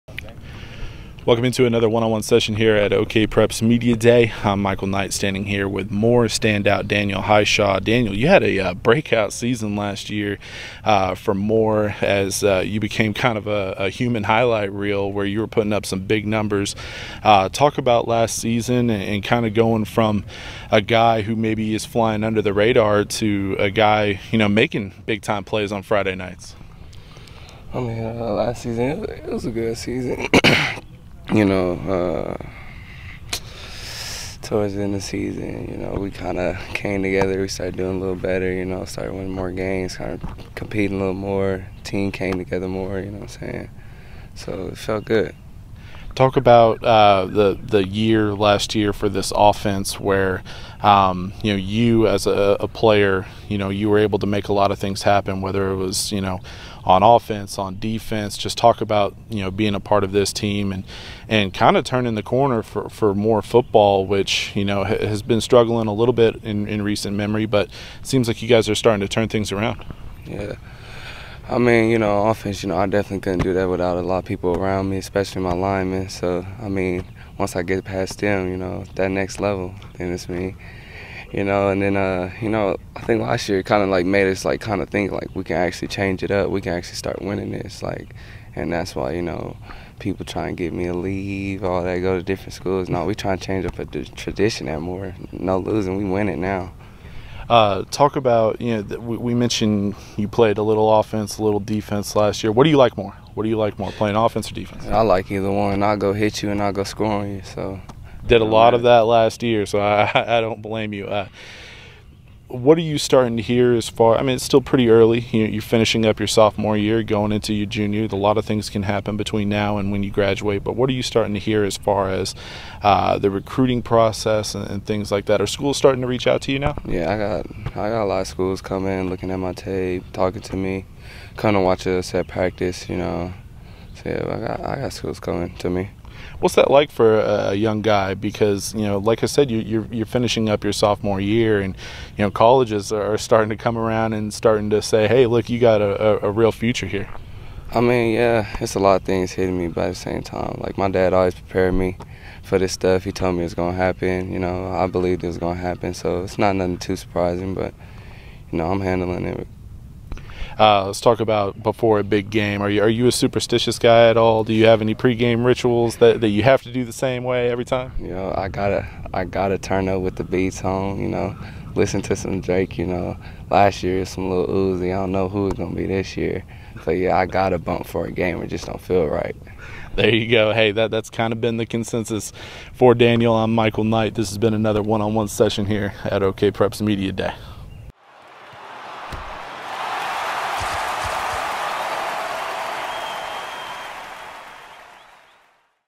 2018 Media Day: Daniel Hishaw
Check out the OKPreps Media Day interview with Moore quarterback Daniel Hishaw.